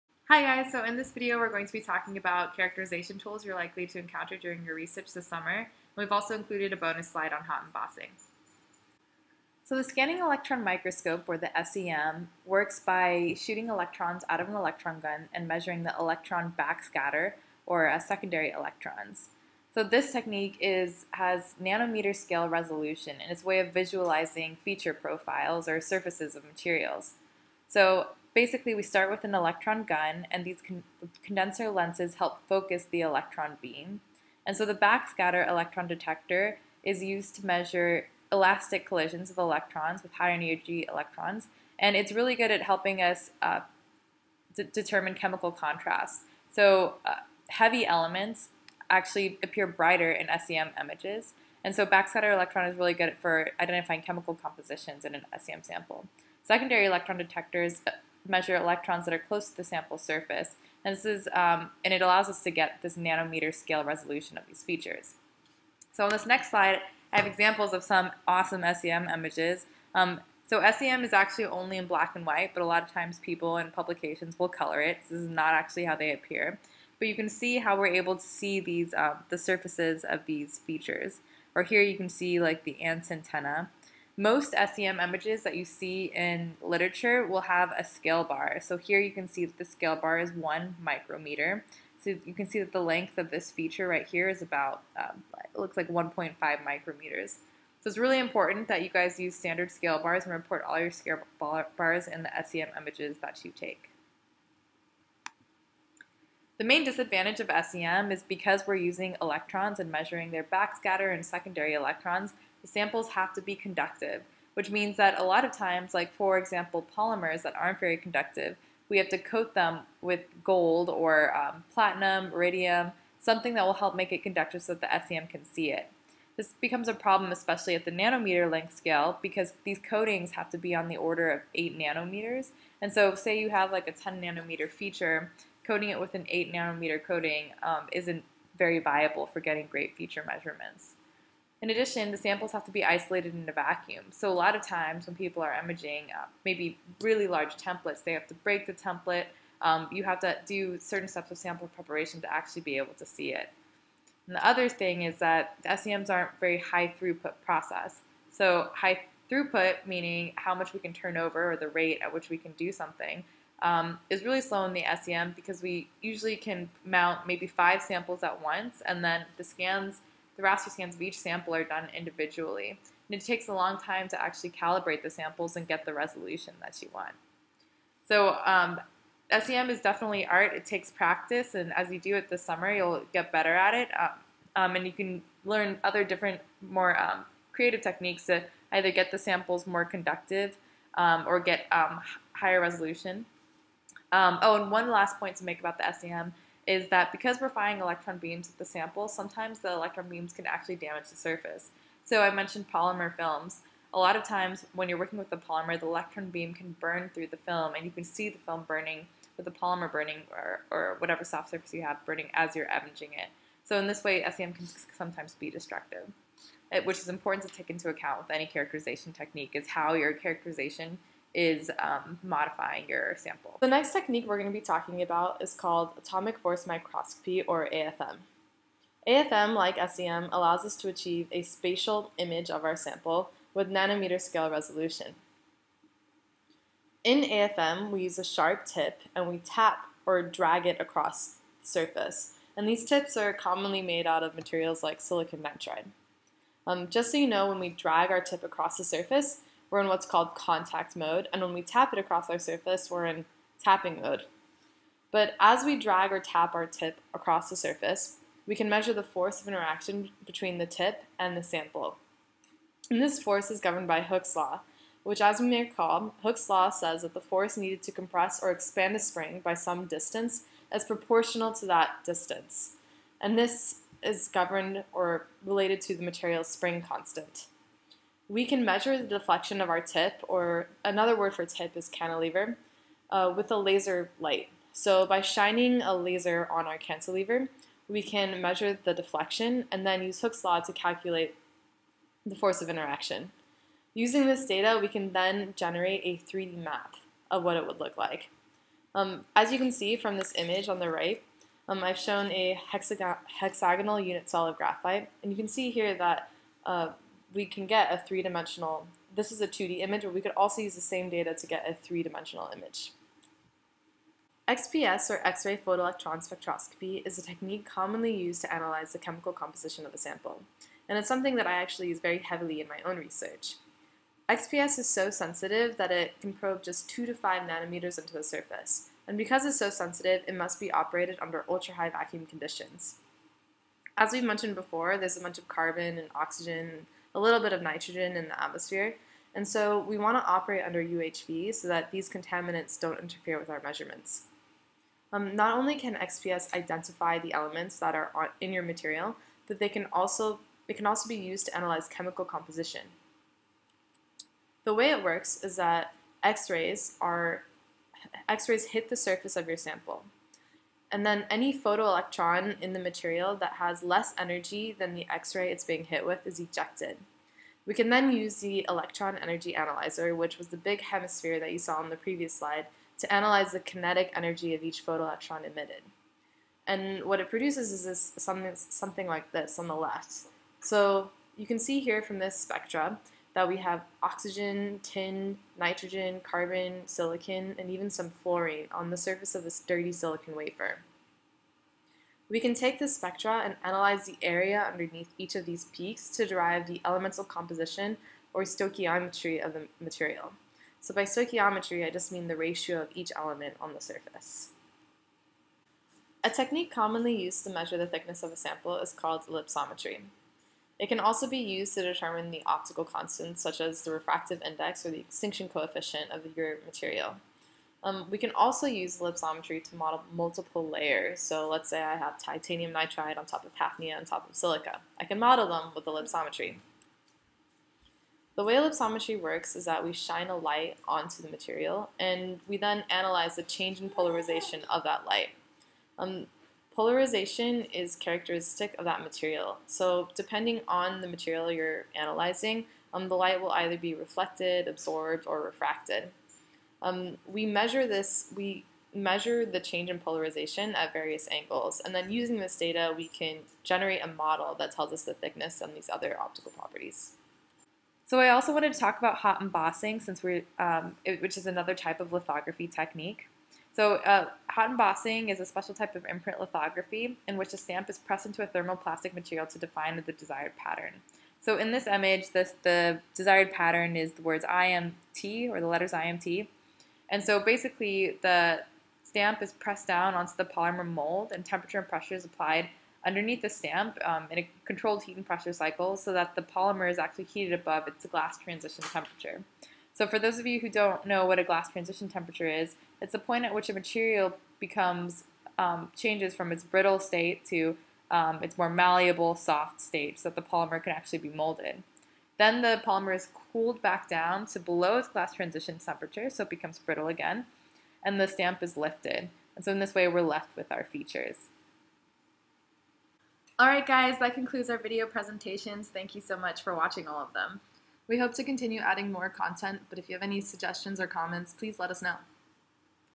NASCENT Boot Camp Lecture 3: Characterization
Table of Contents:

01:06 - Advantages of SEM
01:45 - Disadvantages of SEM
05:53 - How it works
07:07 - Ellipsometry: How it works